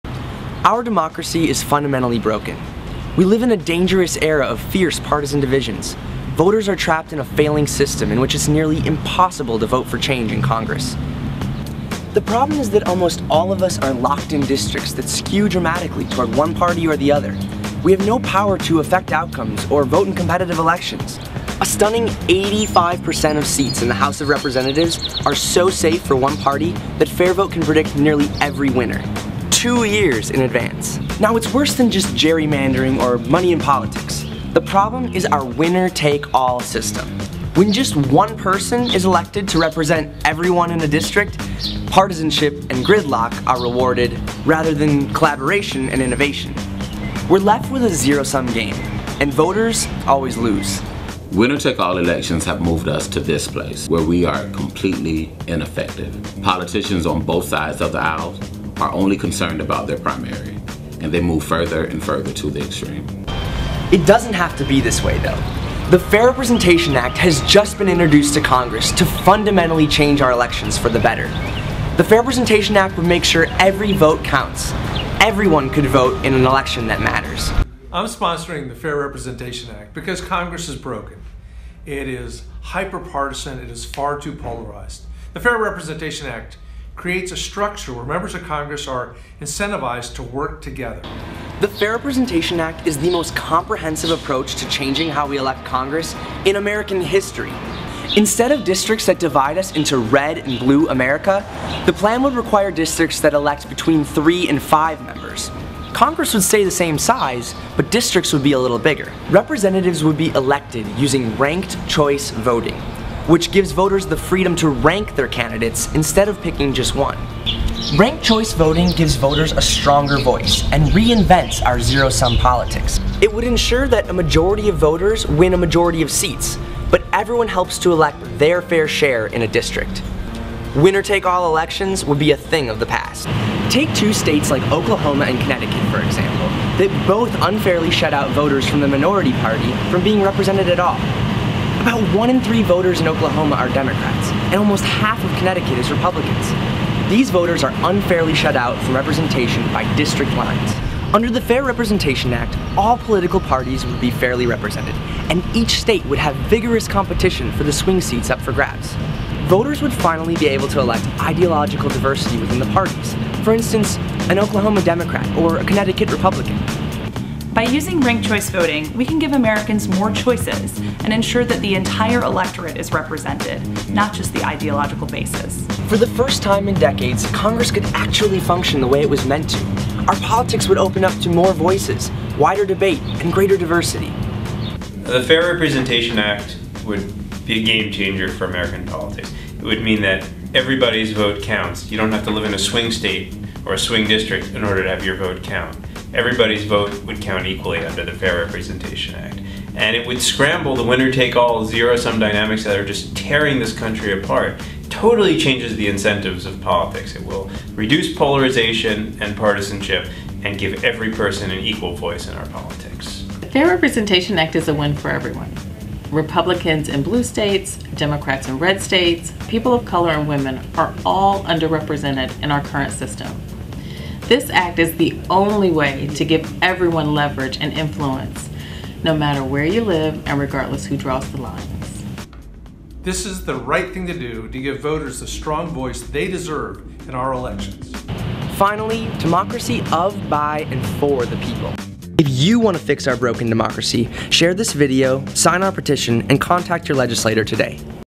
Introducing: The Fair Representation Act
The Fair Representation Act has just been introduced to Congress by Representative Don Beyer (VA-08) and has the potential to fundamentally solve the problems facing our broken politics. to learn more and get involved.